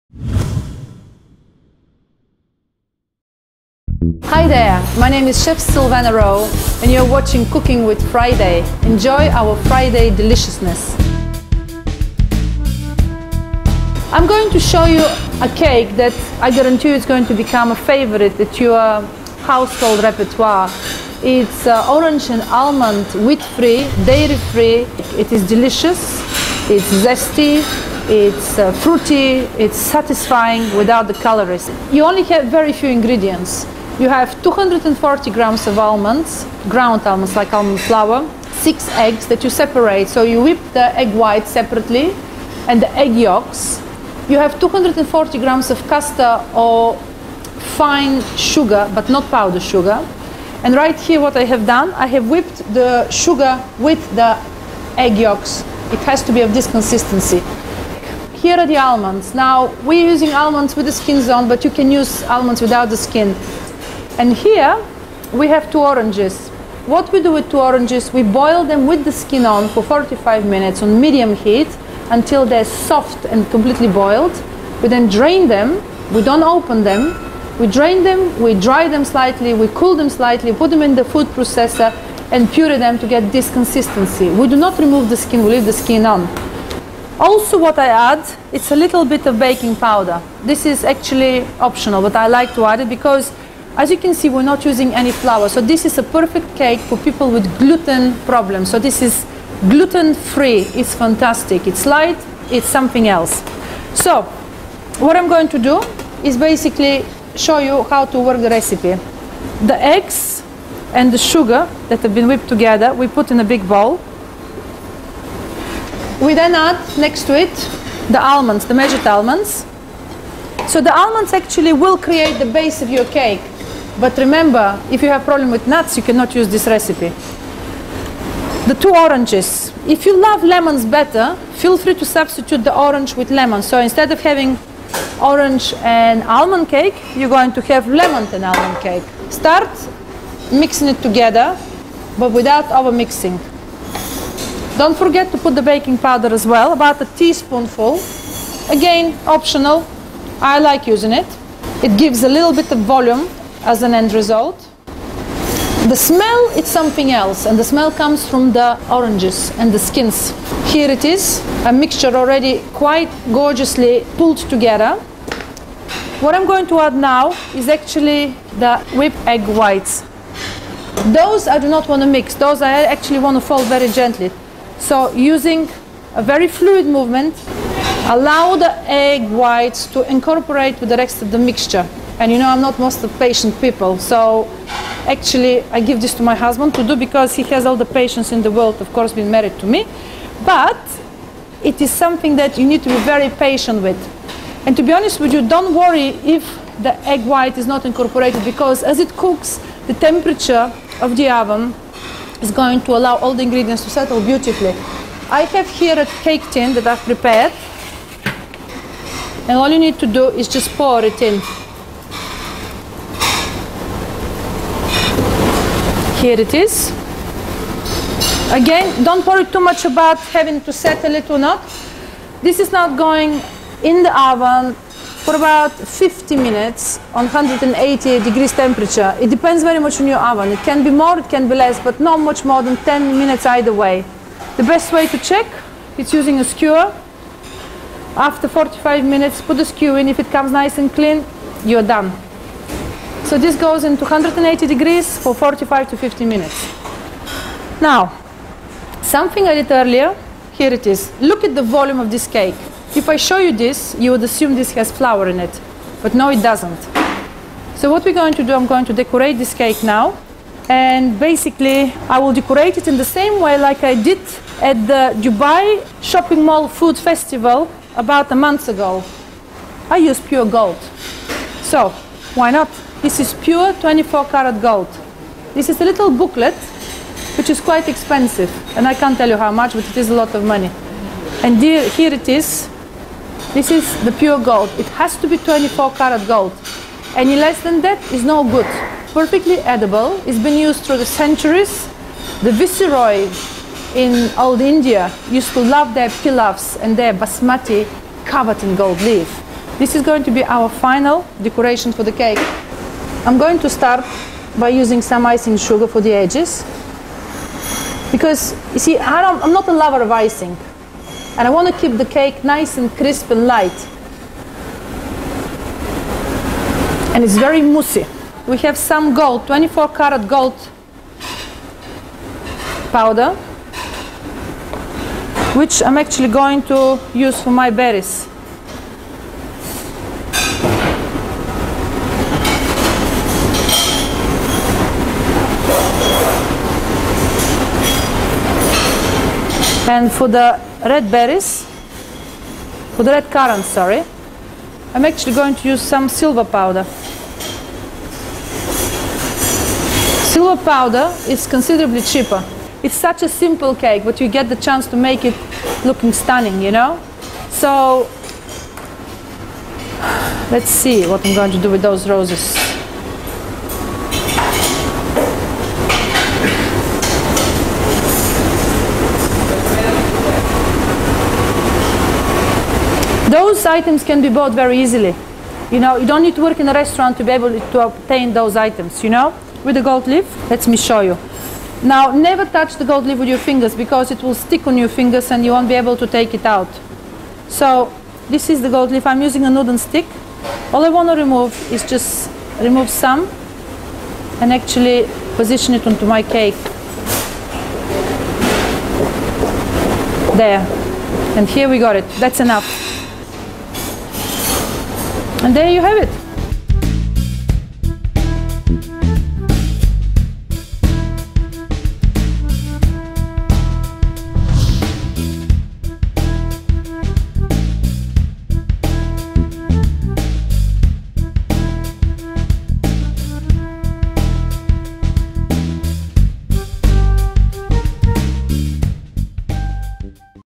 Orange almond cake recipe
Cooking with Friday magazine and Chef Silvena Rowe.

Videographer: Ryan Navarro